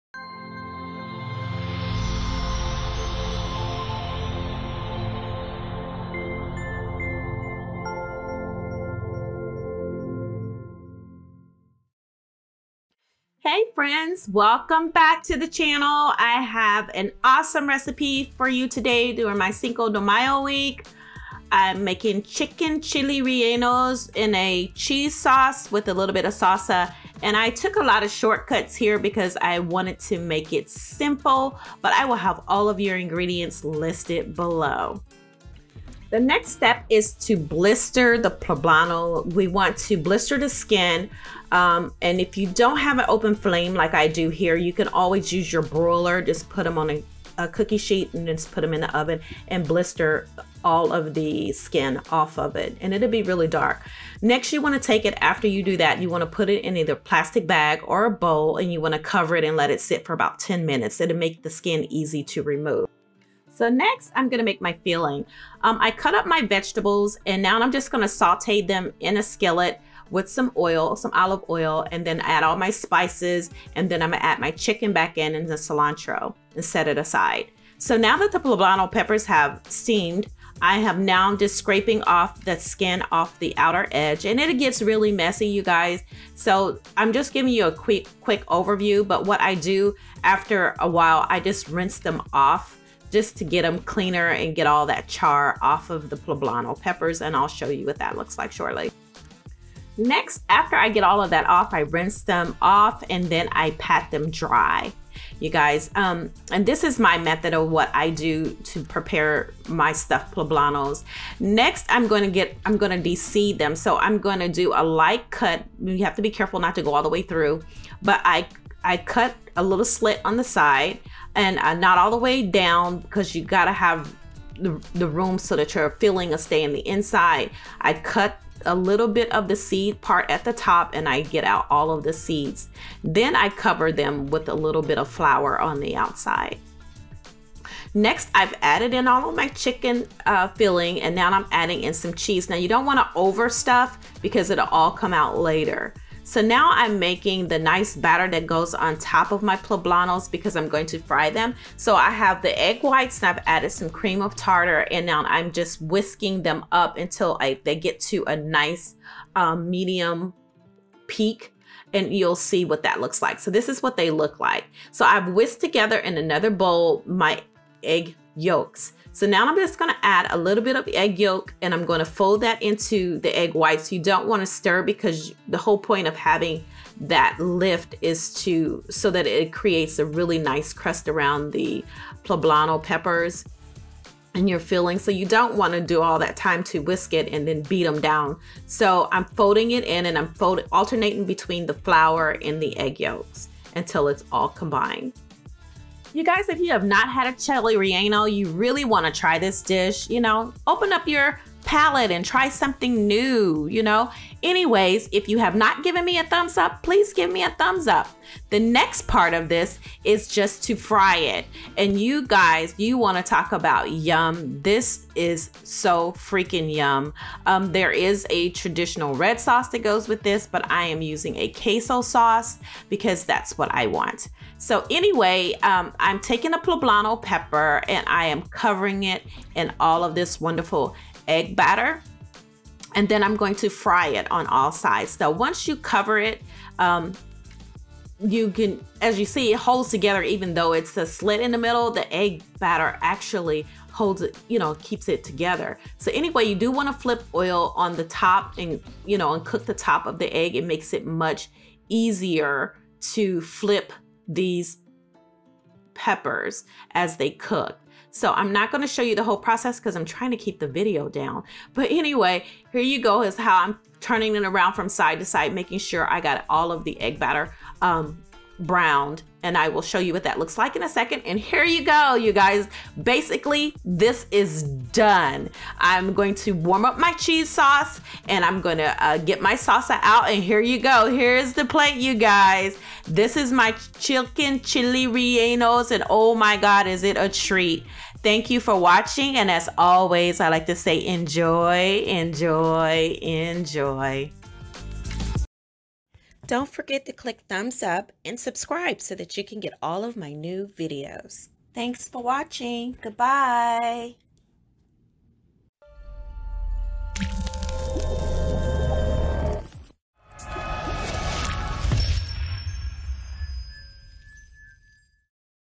Best Chicken Chile Rellenos | Easy Chile Rellenos for Cinco de Mayo! | Stuffed Poblano’s with Queso
This is an authentic Chicken Chile Rellenos that has lots of flavor for this Cinco de Mayo. They are filled with a chicken and vegetables and coated in a delicate, slightly crisp coating and served with a warm queso cheese and salsa. They are super easy to make and you can change up you filling to create an new and exciting dinner!

Ingredients

For Chiles Rellenos
5 poblano peppers
3 cups rotisserie chicken, shredded
3 garlic cloves, chopped
3 tbsp cilantro, chopped extra for topping
½ red onion, sliced
½ bell pepper, sliced
1 ½ tsp. ground cumin
1 tsp. chili powder
1 ½ tsp. granulated garlic
1 ½ tsp granulated onion
1 tsp cayenne blend (optional)
1 ½ tsp sazon caliente seasoning
2 c cheese blend (medium cheddar, monterey jack, queso quesadilla or queso oaxaca or mozzarella)
Olive oil
Queso cheese
Jalapeno
Salsa

For Deep -fry
6 eggs, room temp and whites and yolk separated
¼ c flour
2 c frying oil (I used canola)
½ tsp cream of tartar

For Ranchero Sauce (optional)
1 tbsp. of olive oil
1 yellow onion, small diced
1 garlic clove, minced
1 serrano pepper, seeded and roughly diced
1 ½ tsp dry oregano
1 ½ tsp ground cumin
28 oz. can whole peeled tomatoes

1. In a medium pot over medium heat, add in the oil and cook the onions, garlic and pepper for 3-4 minutes or until lightly browned.
2. Stir in the oregano and cumin, add the tomatoes and cook for an additional 4-5 minutes to infuse the flavors.
3. Season with salt and pepper and blend until completely smooth.